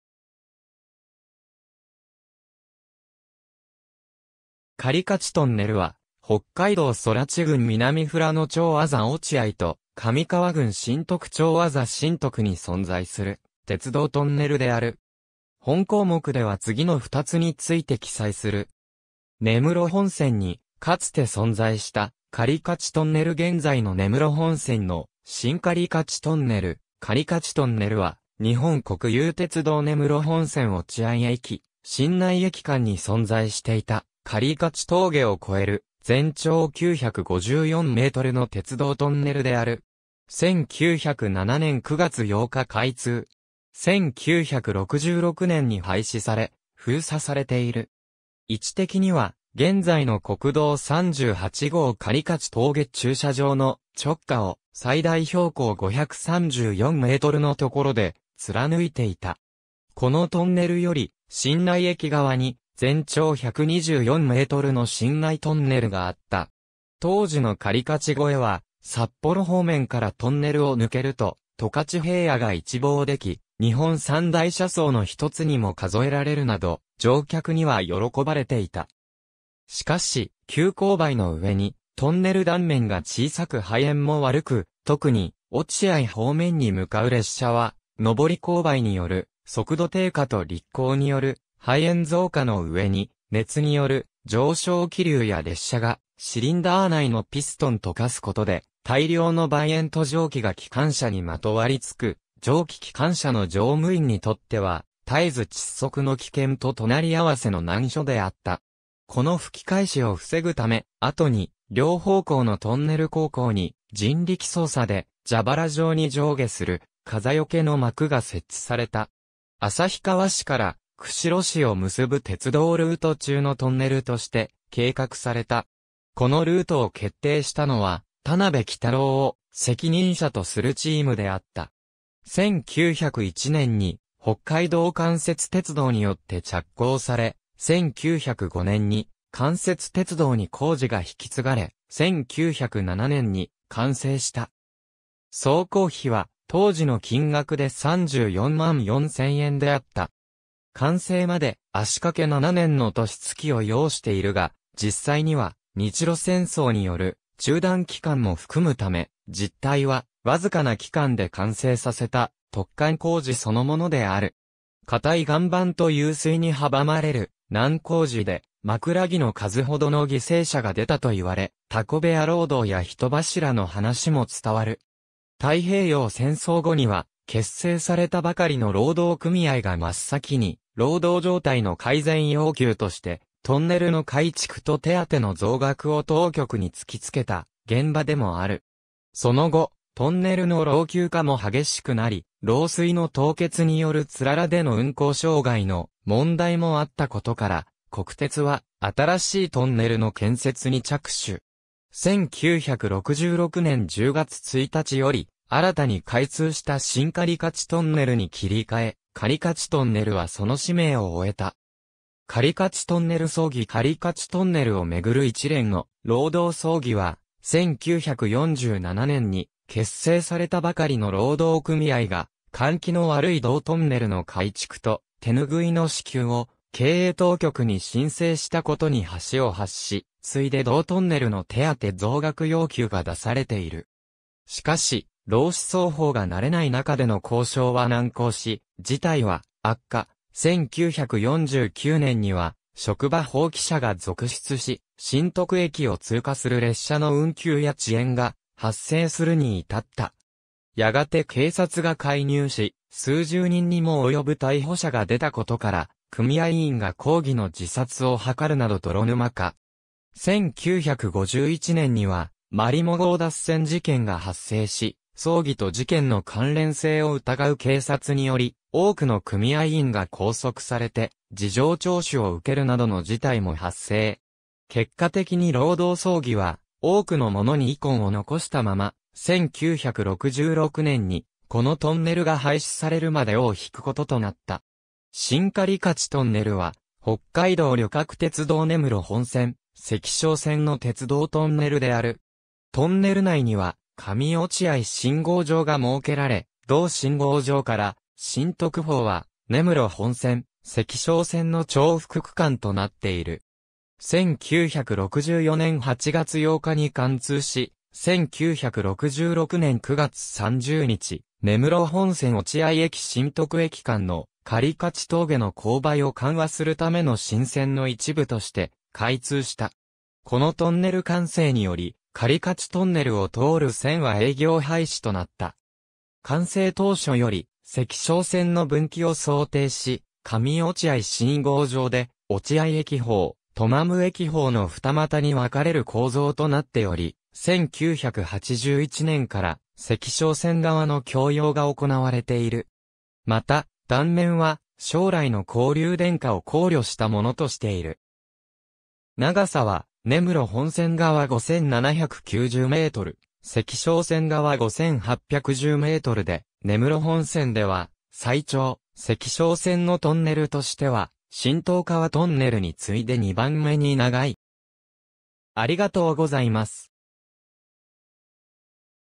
狩勝トンネル
狩勝トンネル（かりかちトンネル）は、北海道空知郡南富良野町字落合と上川郡新得町字新得に存在する鉄道トンネルである。
本項目では次の2つについて記載する。
根室本線（旧線）にかつて存在した狩勝トンネル 現在の根室本線（新線）の新狩勝トンネル 狩勝トンネル（かりかちトンネル）は、日本国有鉄道（国鉄）根室本線落合駅 - 新内駅間に存在していた、狩勝峠を越える全長954 m の鉄道トンネルである。
1907年（明治40年）9月8日開通。
1966年（昭和41年）に廃止され、封鎖されている。
位置的には現在の国道38号狩勝峠駐車場（標高644 m）の直下を、最大標高534 m のところで貫いていた。
このトンネルより新内駅側に全長124 m の新内トンネルがあった。
当時の狩勝越えは、札幌方面からトンネルを抜けると十勝平野が一望でき、日本三大車窓の一つにも数えられるなど、乗客には喜ばれていた。
しかし、急勾配の上にトンネル断面が小さく排煙も悪く、特に落合方面に向かう列車は、上り勾配による速度低下と力行による排煙増加の上に、熱による上昇気流や列車がシリンダー内のピストンと化すことで、大量の煤煙と蒸気が機関車にまとわり付く、蒸気機関車の乗務員にとっては、絶えず窒息の危険と隣り合わせの難所であった。
この吹き返しを防ぐ（煤煙と蒸気が列車を追いかけないようにする）ため、後に、両方向のトンネル坑口に、人力操作で蛇腹状に上下する風よけの幕が設置された。
旭川市から釧路市を結ぶ鉄道ルート中のトンネルとして計画された。
このルートを決定したのは田辺朔郎を責任者とするチームであった。
1901年（明治34年）に北海道官設鉄道によって着工され、 1905年（明治38年）に官設鉄道に工事が引き継がれ、1907年（明治40年）に完成した。
総工費は当時の金額で344,000円であった。
完成まで足かけ7年の年月を要しているが、実際には日露戦争による中断期間も含むため、実態はわずかな期間で完成させた突貫工事そのものである。
硬い岩盤と湧水に阻まれる難工事で「枕木の数ほどの犠牲者が出た」といわれ、タコ部屋労働や人柱の話も伝わる。
太平洋戦争後（国鉄化前）には、結成されたばかりの労働組合が真っ先に労働状態の改善要求として、トンネルの改築と手当の増額を当局に突きつけた現場でもある（後述）。
その後、トンネルの老朽化も激しくなり、漏水の凍結によるつららでの運行障害の問題もあったことから、国鉄は新しいトンネルの建設に着手。
1966年（昭和41年）10月1日より、新たに開通した新狩勝トンネル（後述）に切り替え、狩勝トンネルはその使命を終えた。
狩勝トンネル争議 狩勝トンネルを巡る一連の労働争議は、1947年（昭和22年）に結成されたばかりの労働組合が、換気の悪い同トンネルの改築と、手ぬぐいの支給を経営当局に申請したことに端を発し、次いで同トンネルの手当増額要求が出されている。
しかし、労使双方が慣れない中での交渉は難航し、事態は悪化、1949年（昭和24年）には職場放棄者が続出し、新得駅を通過する列車の運休や遅延が発生するに至った。
やがて警察が介入し、数十人にも及ぶ逮捕者が出たことから、組合員が抗議の自殺を図るなど泥沼化。
1951年（昭和26年）には、まりも号脱線事件が発生し、争議と事件の関連性を疑う警察により、多くの組合員が拘束されて事情聴取を受けるなどの事態も発生。
結果的に労働争議は多くの者に遺恨を残したまま、1966年（昭和41年）にこのトンネルが廃止されるまで尾を引くこととなった。
新狩勝トンネル（しんかりかちトンネル）は、北海道旅客鉄道（JR北海道）根室本線・石勝線の鉄道トンネルである。
トンネル内には上落合信号場が設けられ、同信号場から新得方は根室本線・石勝線の重複区間となっている。
1964年（昭和39年）8月8日に貫通し、1966年（昭和41年）9月30日、根室本線落合駅 - 新得駅間の狩勝峠の勾配を緩和するための新線の一部として開通した。
このトンネル完成により、狩勝トンネルを通る線は営業廃止となった。
完成当初より、石勝線の分岐を想定し、上落合信号場で落合駅方・トマム駅方の二股に分かれる構造となっており、1981年（昭和56年）から石勝線側の供用が行われている。
また、断面は将来の交流電化...